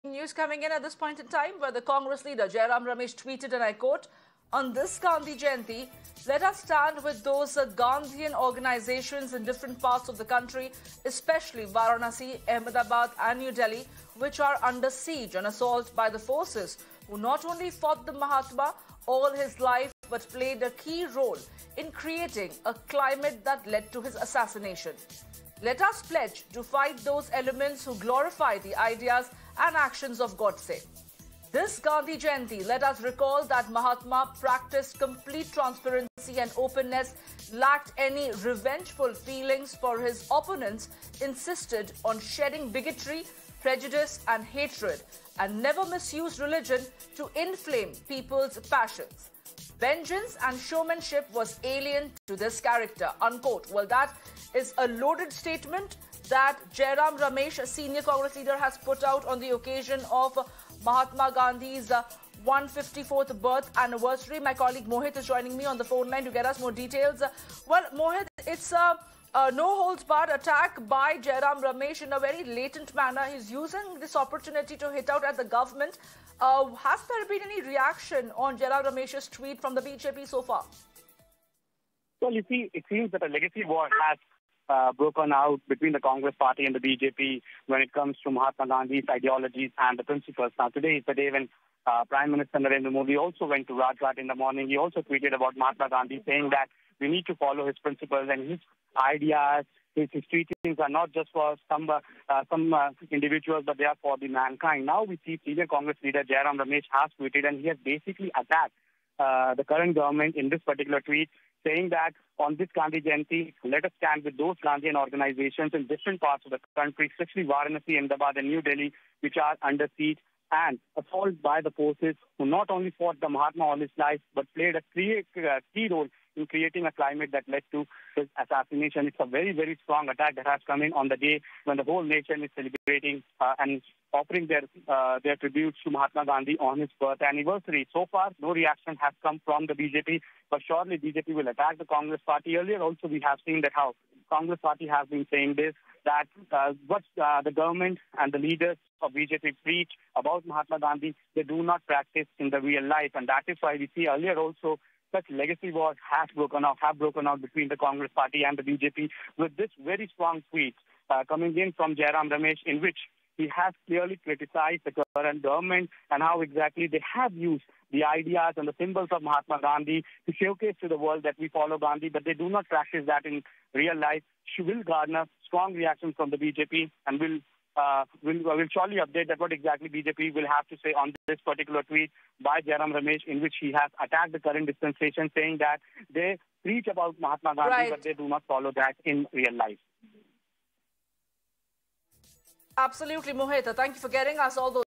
'Fight Those Who Glorify Godse,' Jairam Ramesh Jibe At BJP Over Gandhi Jayanti | English News
Taking to Twitter, Congress leader Jairam Ramesh slammed the BJP government on Gandhi Jayanti. He wrote, "On this Gandhi Jayanti, let us stand with those Gandhian organisations in different parts of the country, especially Varanasi, Ahmedabad and New Delhi which are under seige and assault by the forces who not only fought the Mahatma all his life but played a key role in creating a climate that led to his assassination. Let us pledge to fight those elements who glorify the ideas and actions of Godse. This Gandhi Jayanti, let us expose the total and complete hypocrisy of those who appropriate Gandhian symbols — his spectacles, charkha and walking stick — and project his legacy to the world."